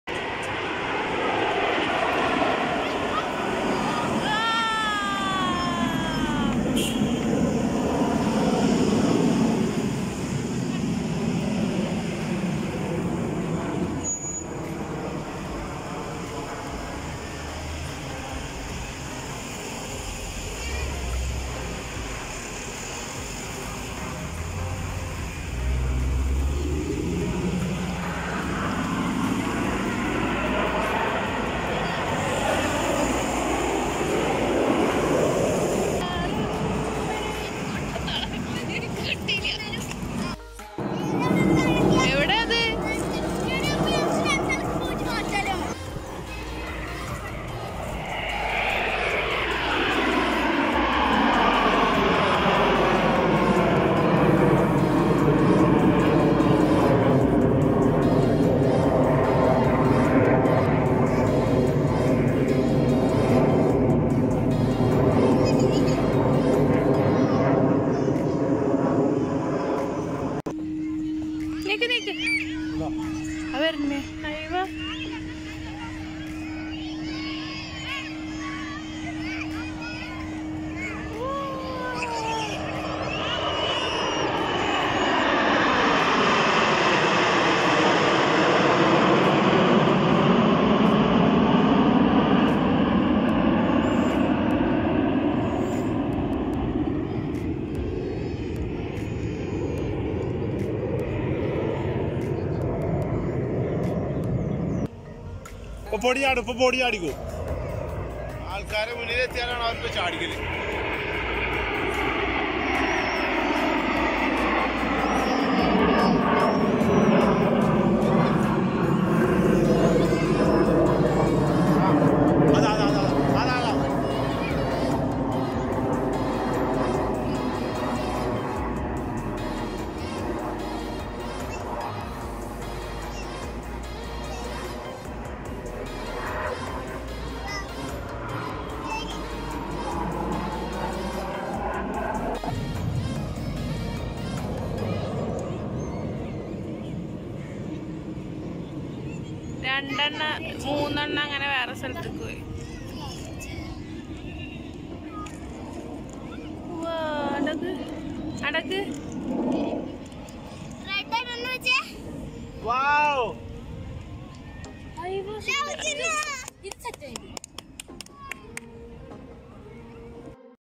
khorfakkan beach ന് പ്രതീക്ഷിക്കാതെ കാണാൻ പറ്റിയ Air show#airshow#khorfakkanbeach@ Rayya's world
khorfakkan beach le air show